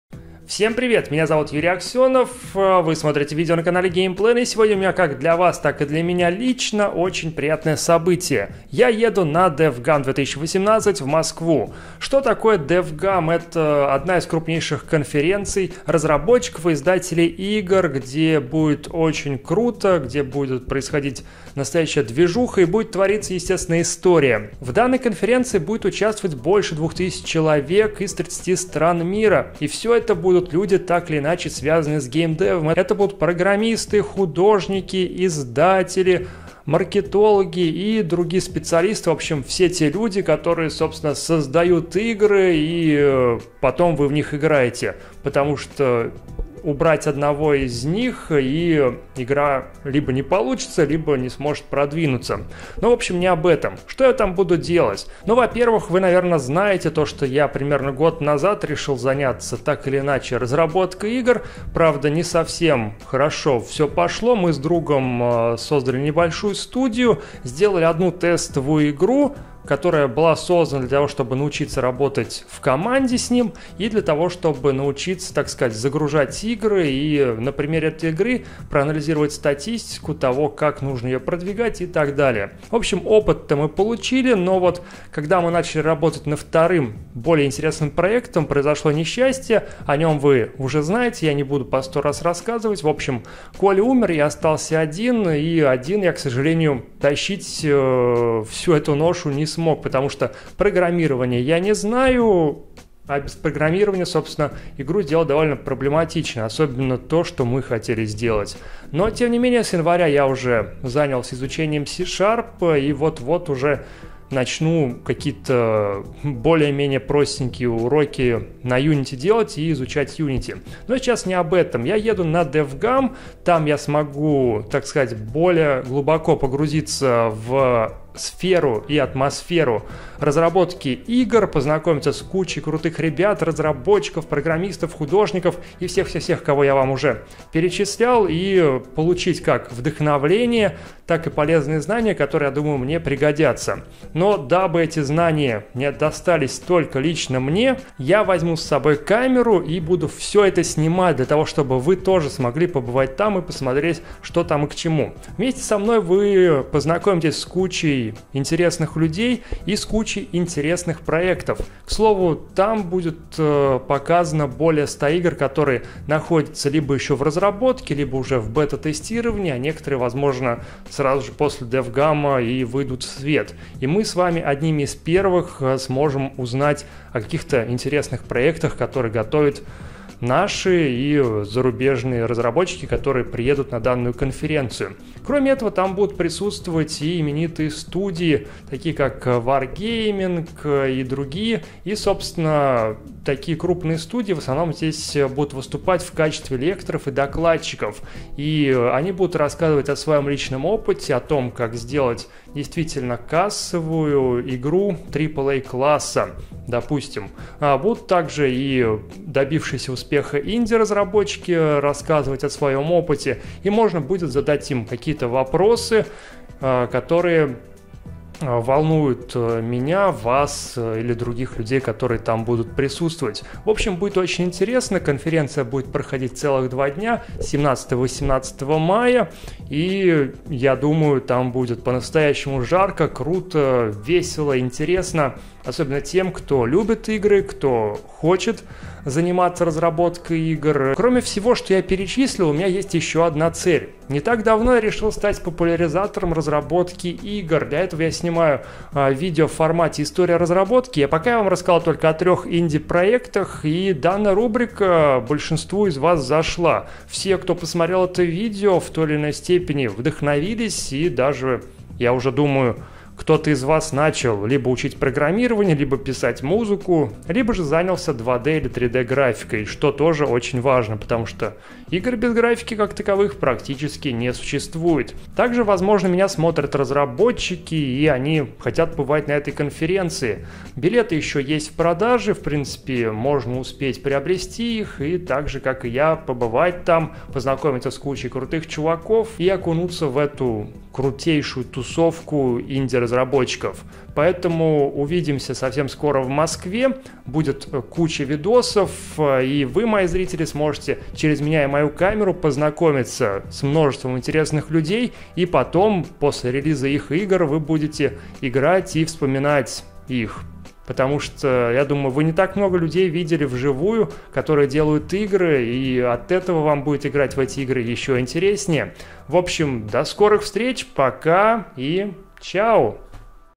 Я ЕДУ НА DEVGAMM MOSCOW 2018
Я еду на DevGAMM Moscow 2018. DevGAMM — это крупнейшая конференция для профессионалов игровой индустрии в России, Украине и Беларуси. Преимущества DevGAMM: качественный контент на тему разработки игр, низкий порог входа для начинающих разработчиков и серия мероприятий, нацеленных на продуктивное времяпровождение и эффективный нетворкинг.